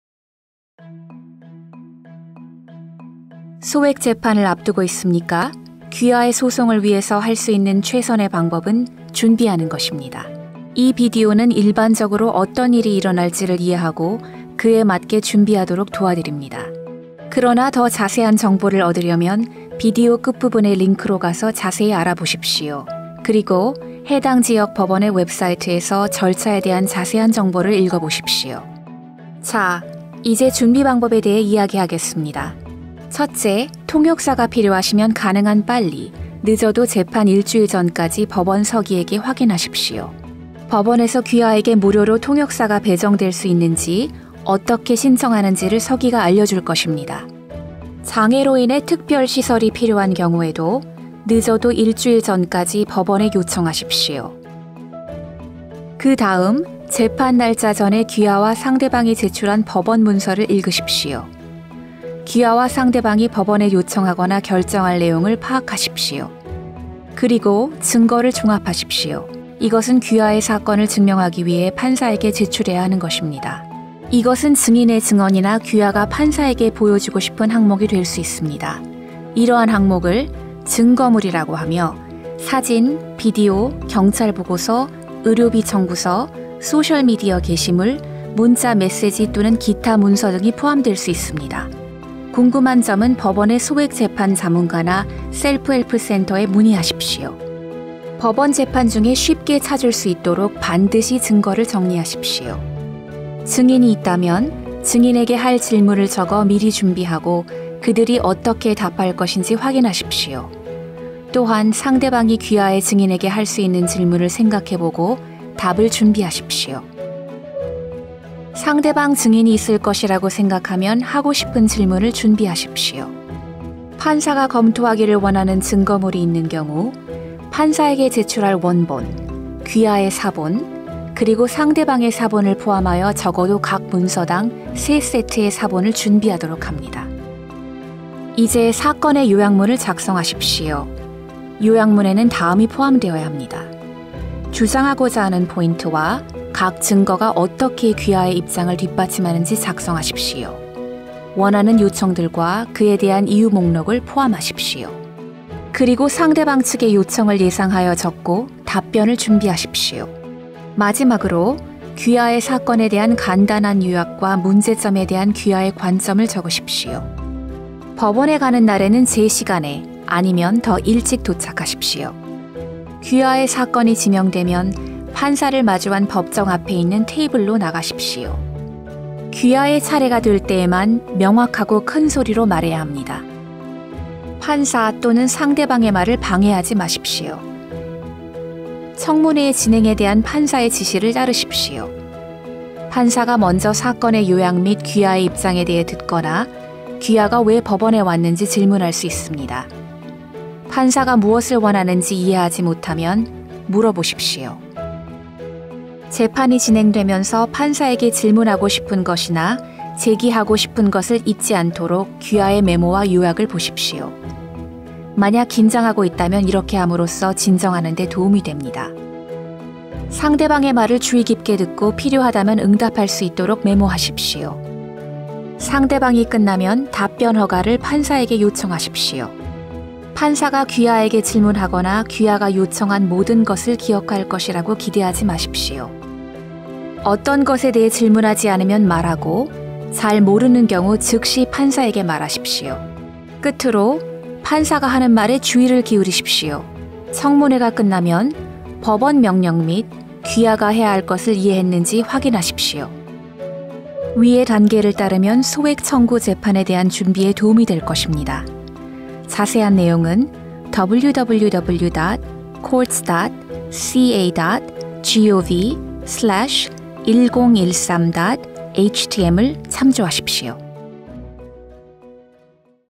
Preparing for Your Small Claims Trial [Korean]
Learn how to prepare your questions, evidence, notes, and summary of events before you go to small claims court.

for an online program that will provide information about small claims cases. This information can help you decide if small claims is right for you, show you how to resolve your case without going to court, and more. [Korean]